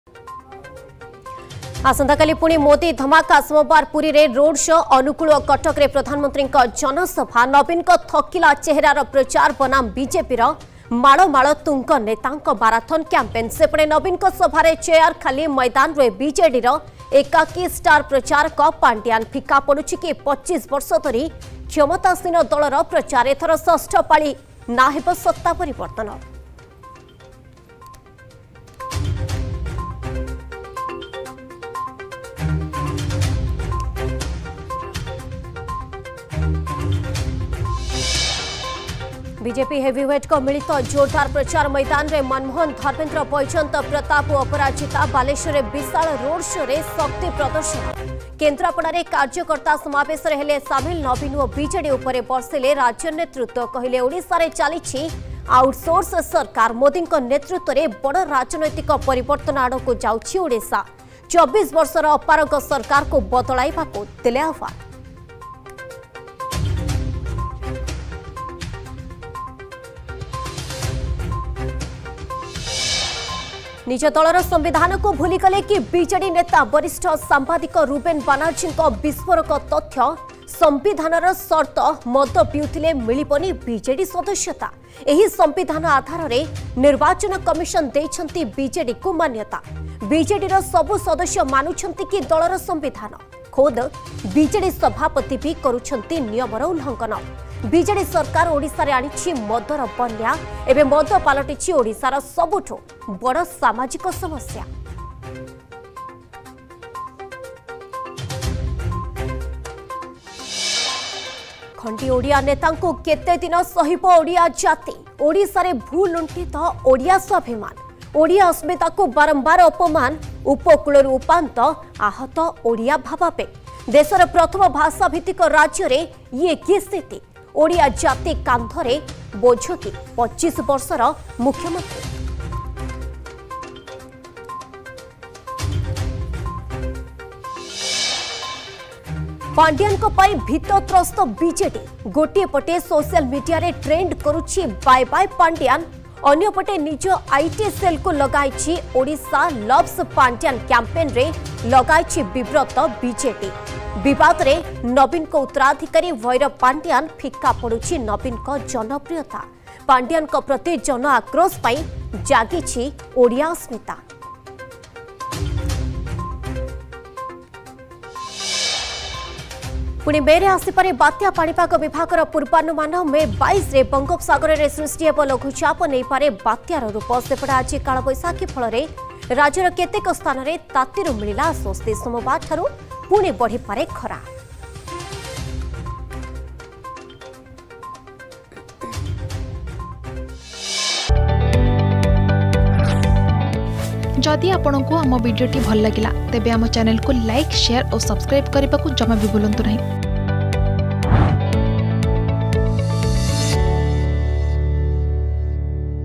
Headlines 7pm || 18 May 2024 || ArgusNews
+ଆସନ୍ତାକାଲି ପୁଣି ମୋଦି ଧମାକା
+ବିଜେପି ହେଭିୱେଟଙ୍କ ମିଳିତ ଜୋରଦାର ପ୍ରଚାର
+ନିଜ ଦଳର ସମ୍ବିଧାନକୁ ଭୁଲିଗଲେ କି ବିଜେଡି ନେତା
+ଖଣ୍ଡି ଓଡ଼ିଆ ନେତାଙ୍କୁ କେତେ ଦିନ ସହିବ ଓଡ଼ିଆ ଜାତି